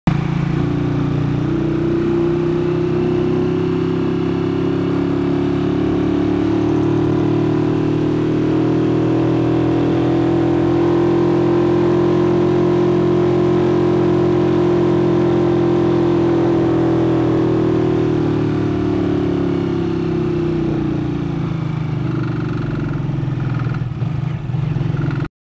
Streets of Boracay, Philippines 2/3
Riding in a taxi in Boracay